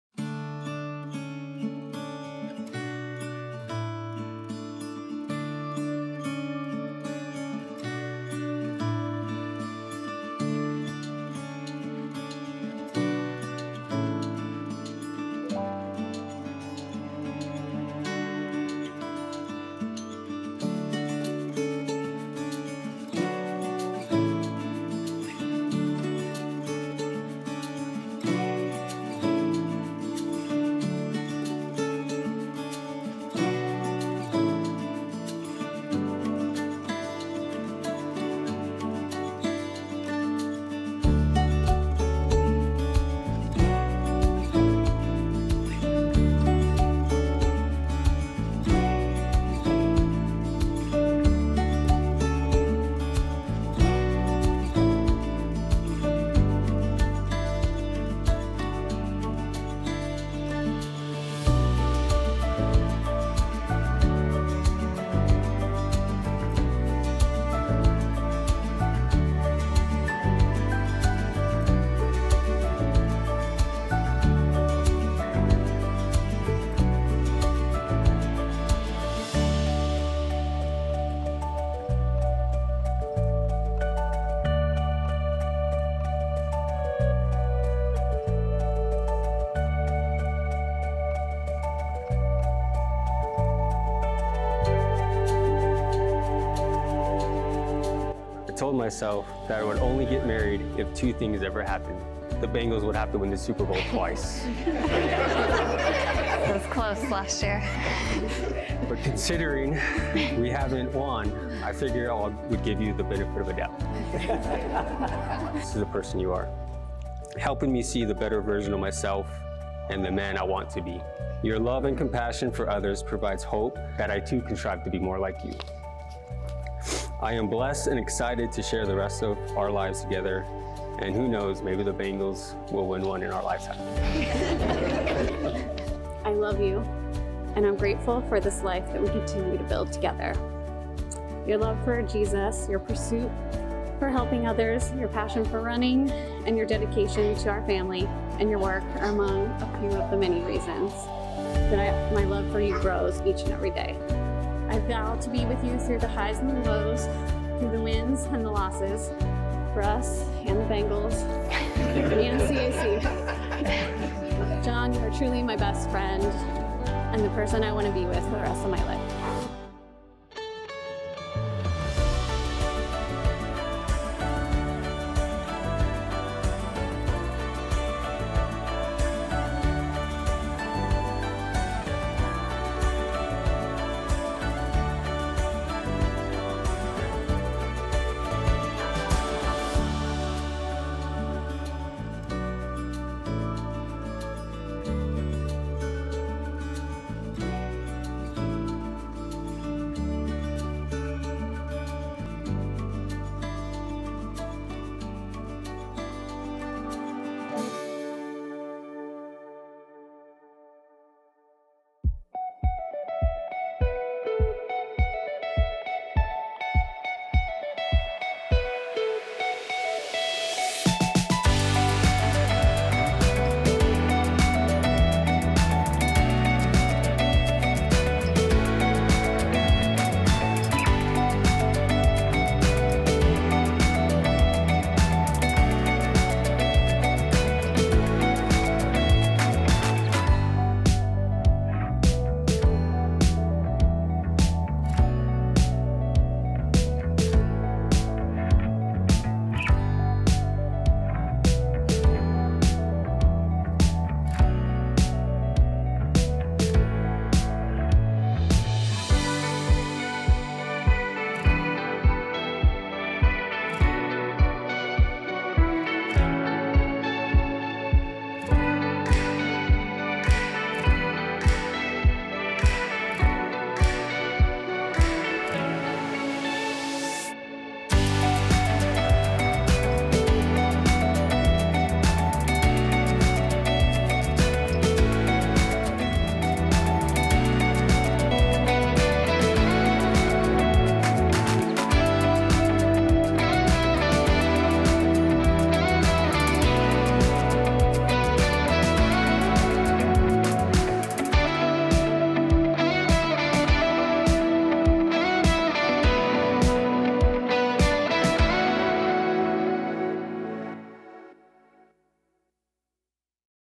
Shayna and Jon Highlight
I was so lucky to be there when Shayna first met Jon. I'll even take credit for immediately telling her to go for it after he gave her his card at a cross country meet! These two runners had a beautiful wedding day in Scottsdale. It was such a delight to be there for their special day and to witness the sweet union of their family.